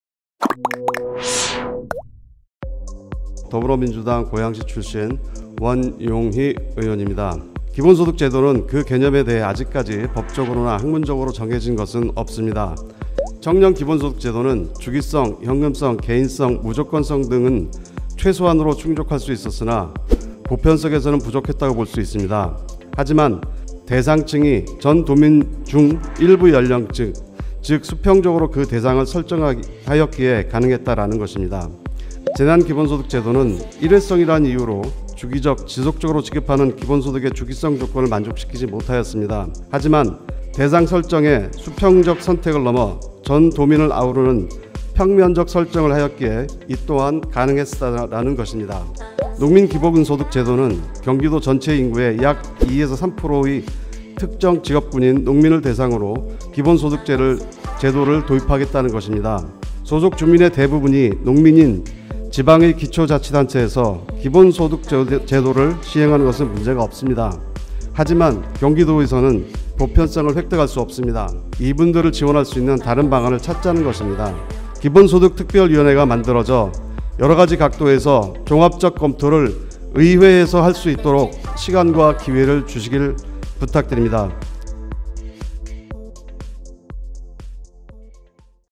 기본소득전문가가 이야기해드립니다! 경기도 농민기본소득 반대 이유!
제344회 정례회 본회의에서의 5분 자유발언!
원용희(고양5)의원의 "경기도 농민기본소득 조례안 반대이유" 등을
시청해 주세요~!